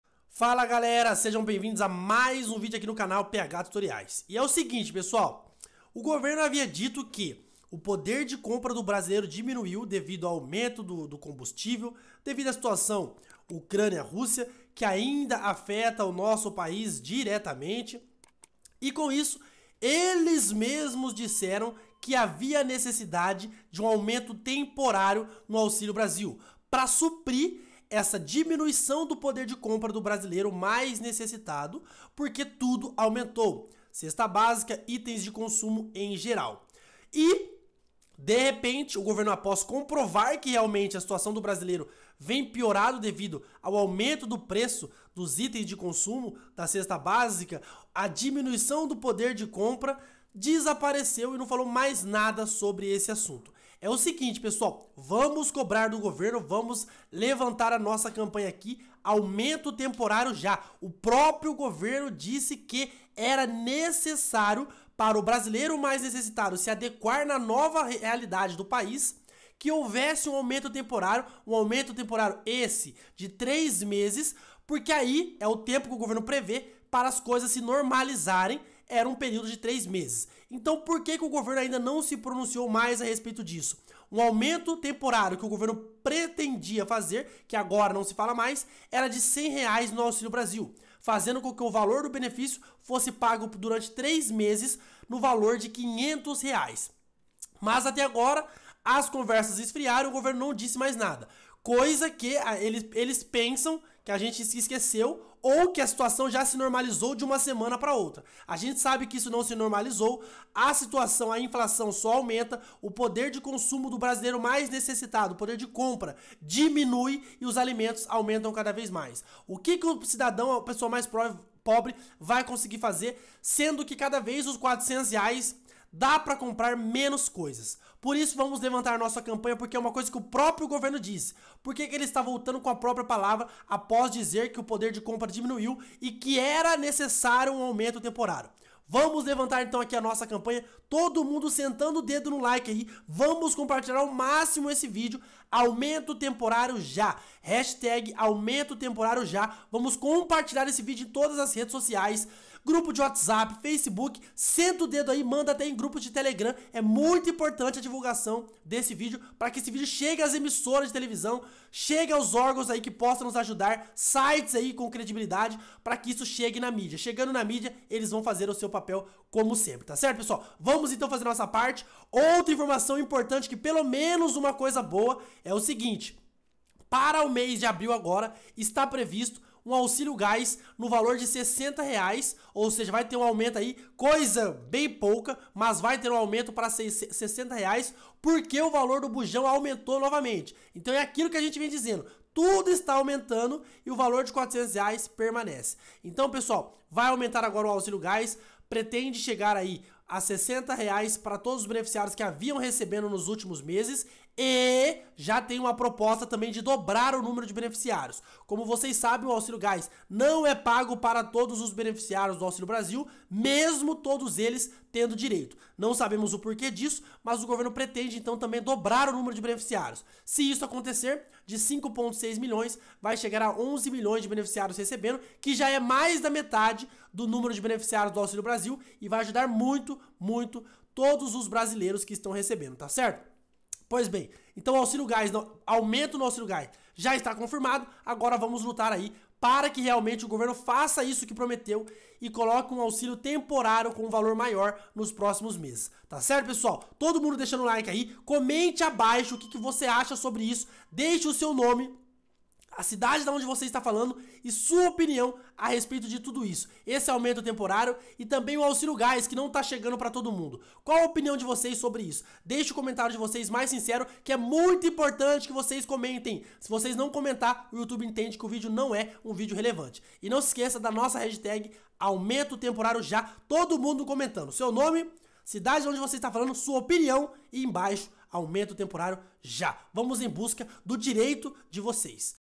AUMENTO NO AUXÍLIO BRASIL PARA R$500... VALOR MAIOR POR 3 MESES
Ministério da Economia avalia elevação do valor do Auxílio Brasil para 500 reais por 3 meses.